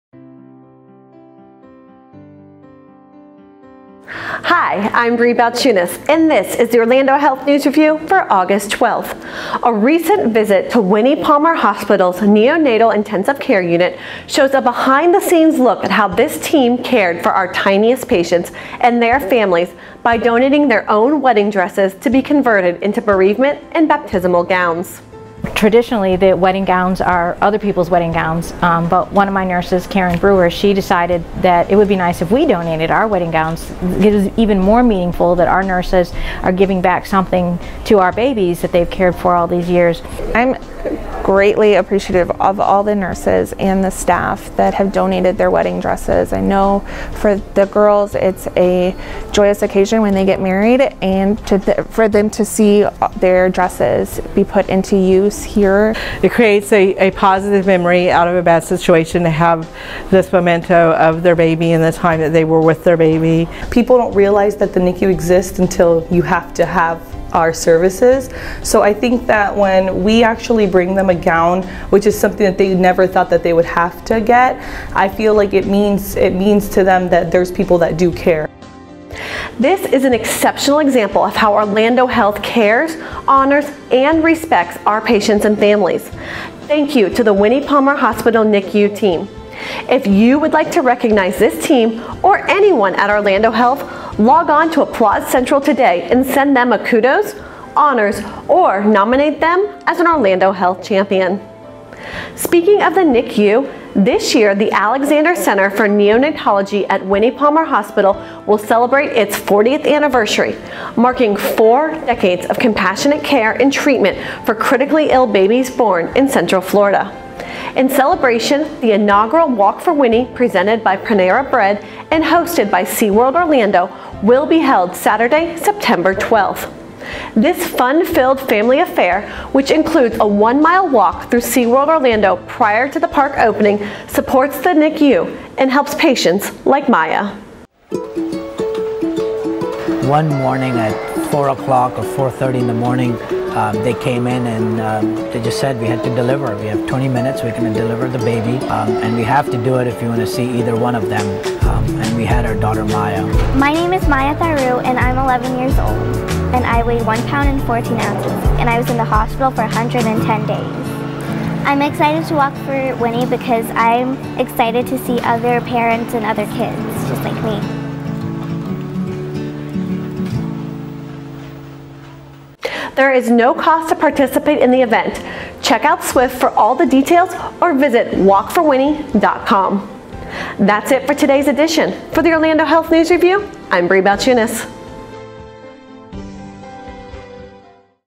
Orlando Health News Review, Episode 96
In this episode, hear about how nurses in the NICU at Winnie Palmer Hospital for Women & Babies are caring for our tiniest patients and their families by donating their own wedding dresses to be converted into bereavement and baptismal gowns. Also, get information about the inaugural Walk for Winnie, presented by Panera Bread and hosted by SeaWorld Orlando.